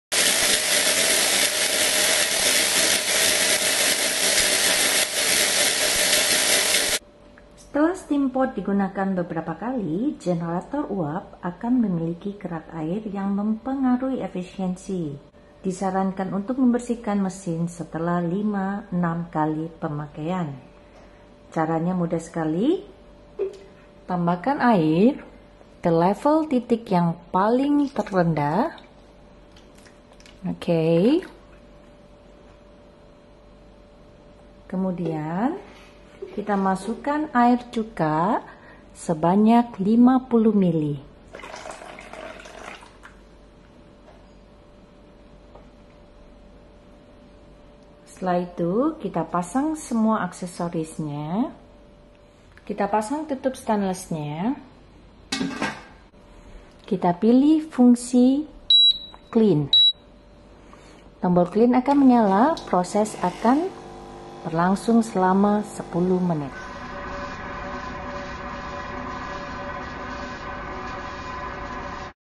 Steam Cooking Signora
Cara membersihan Steam Cooking Signora

steam cooking pot dapat *menghasilkan uap dengan cepat, hanya hitungan detik *

Pastikan menginfo cara pembersihan POMPA dengan air cuka untuk MEMBERSIHKAN kerak air yg menempel pada alat.

info produk dan orderan hanya di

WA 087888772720